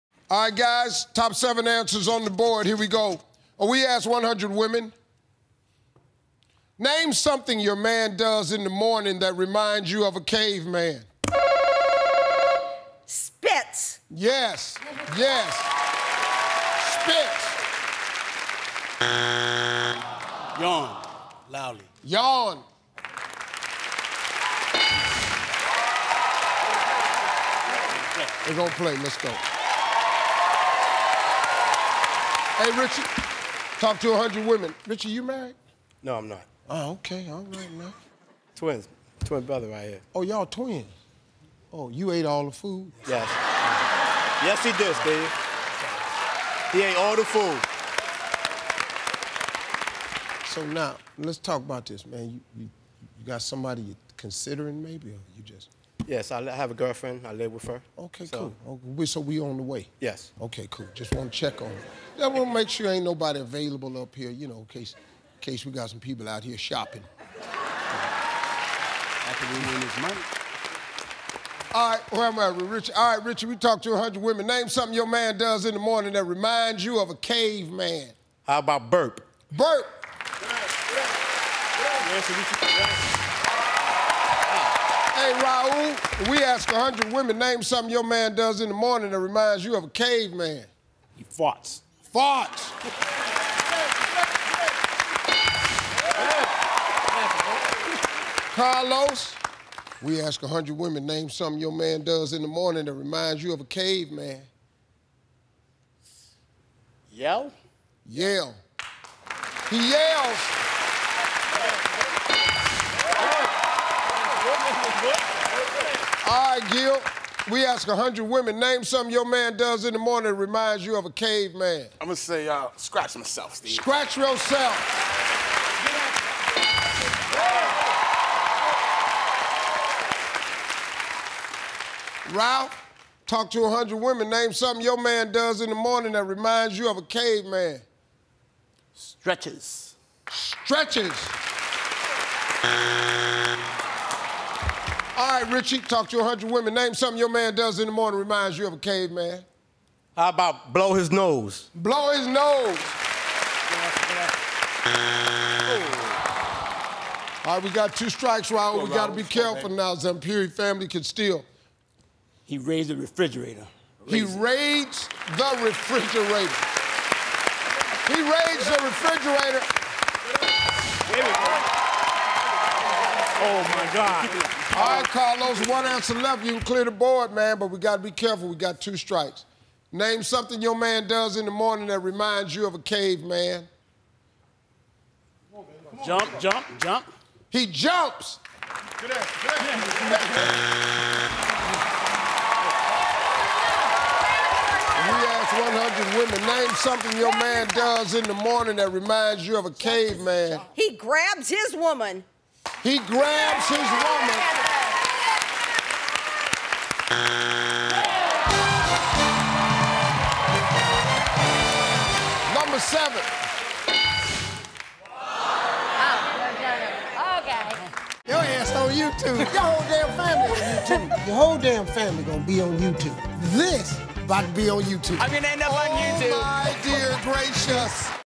Here's why my man is like a caveman...
We asked 100 women... Name something your man does in the morning that reminds you of a caveman...